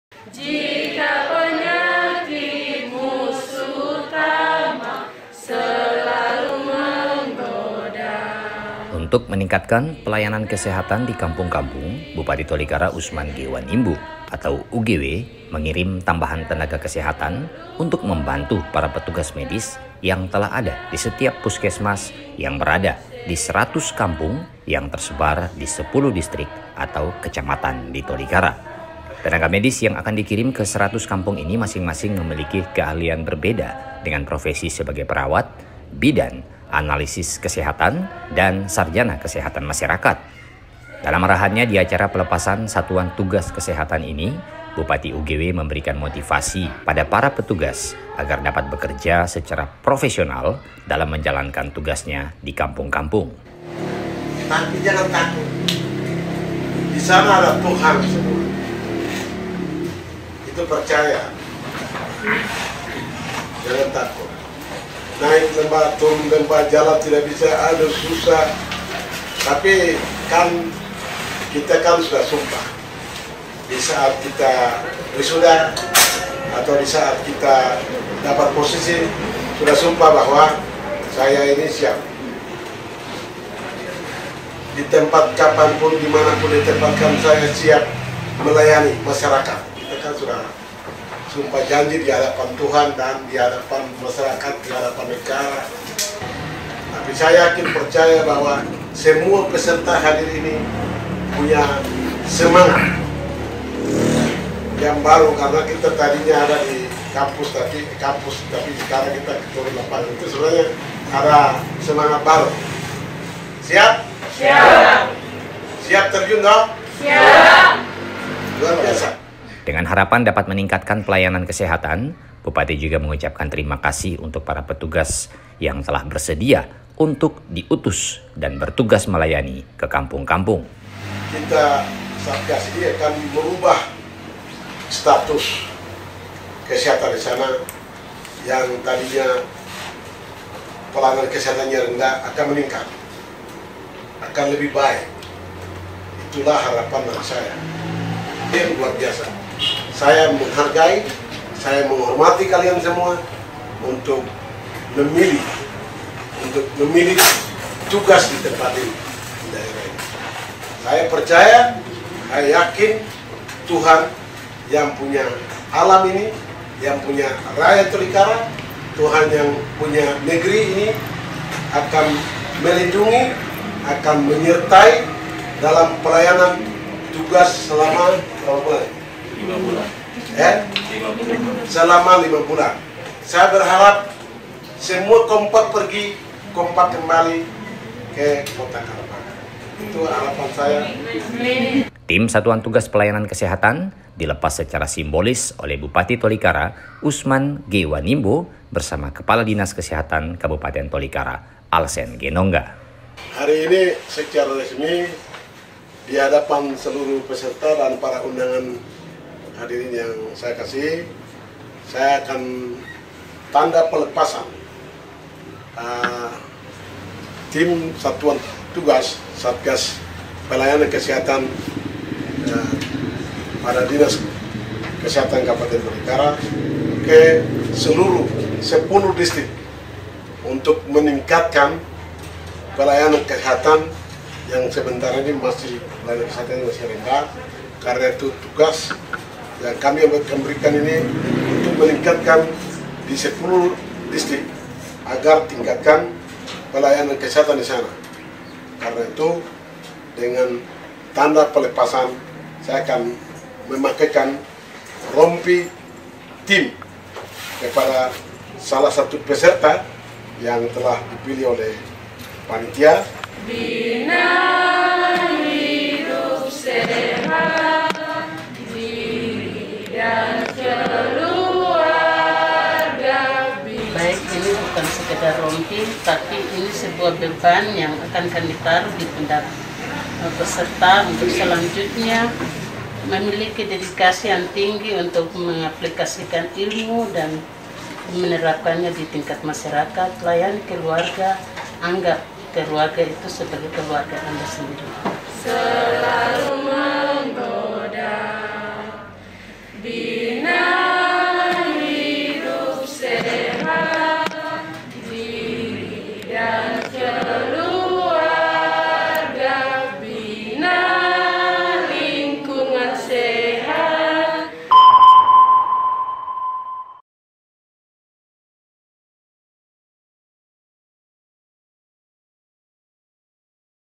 Pelepasan Tenaga Medis ke 100 Kampung Wilayah Pedalaman Tolikara
VIDEOPAPUA.TV - Untuk meningkatkan Pelayanan Kesehatan di Kampung Kampung, Bupati Tolikara Usman G Wanimbo atau UGW mengirim tambahan tenaga kesehatan untuk membantu para petugas  medis yang telah ada di setiap puskesmas yang berada di 100 kampung yang tersebar di 10 distrik